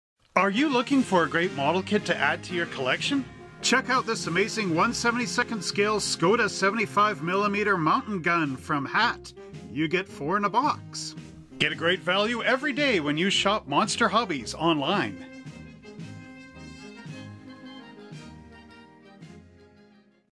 Monster Hobbies - HAT 1/72nd Scale Skoda 75mm Mountain Gun Commercial
Hat 1-72 WWI Skoda 75mm Mountain Gun
This is an unfinished plastic model kit
Shrink wrapped
Paint and cement not included
This set includes :
4 Guns
Awesome for dioramas or war games!

From Wikipedia :

The Skoda 7.5 cm Gebirgskanone M. 15 was a mountain gun used by Austria-Hungary in World War I. In German service, it was known as the 7.5 cm GebK 15. The Italians designated them as the Obice da 75/13 and the Wehrmacht would designate captured guns as 7.5 cm GebK 259(i) after the surrender of Italy in 1943.

Its development was quite prolonged, as the Austrians couldn't decide on the specifications that they wanted. Initially, they wanted a gun that could be broken down into no more than five pack-animal loads to replace the various 7 cm mountain guns in service, but prolonged trials proved that the 7.5 cm M. 12 prototype to be the best gun. However, the commander-in-chief of Bosnia-Hercegovina believe it to be too heavy and demanded a return to the 7 cm caliber to save weight. Skoda dutifully built enough guns for a test battery in the smaller caliber and tested them during the spring of 1914 where they were judged inferior to the 7.5 cm guns. This cost the Austrians heavily as the 7.5 cm guns began to be delivered in April 1915 instead of the planned date of April 1914.

For transport, the gun could be dismantled into six parts, generally carried in four loads. In addition, there was a gun shield fitted on some (perhaps many) such guns. A revised version of this gun was released as the Skoda 75 mm Model 1928.